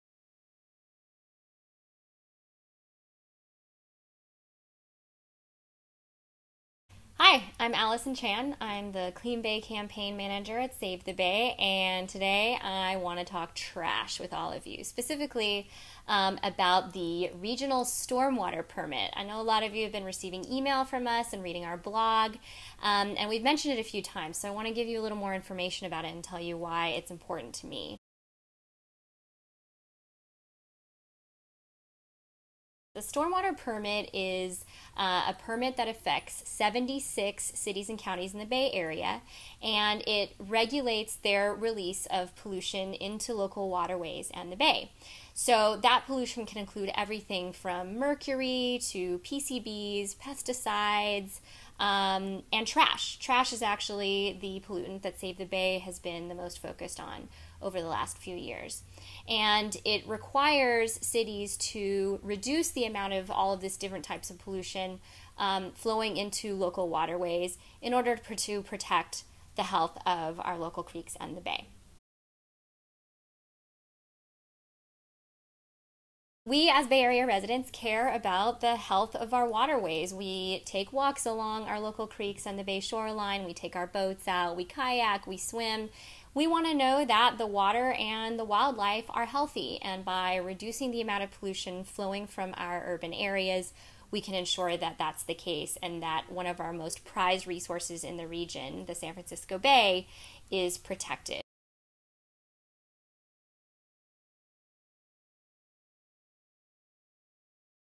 What is the Regional Stormwater Permit? And how does it impact you?
The Regional Stormwater Permit requires 76 Bay Area cities and counties to reduce the amount of pollution flowing into local creeks and the Bay from our Urban Areas. Pollution, also known as urban runoff, includes a wide variety of materials and chemicals, like mercury, pesticides, PCBs, and trash. All of this pollution enters the Bay through the stormwater system,  which enters San Francisco Bay by way of municipal stormwater systems.

At this moment we have a chance to call on the Waterboard to improve the permit by demanding stronger stormwater pollution regulations.

TAKE ACTION NOW and do a good deed for San Francisco Bay